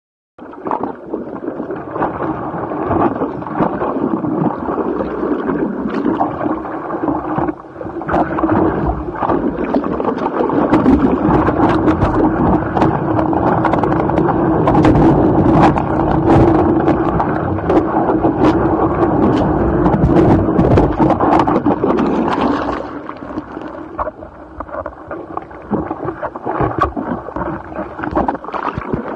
surfing winter storm stella 2 for editing
Kenny Bernstein surfing south beach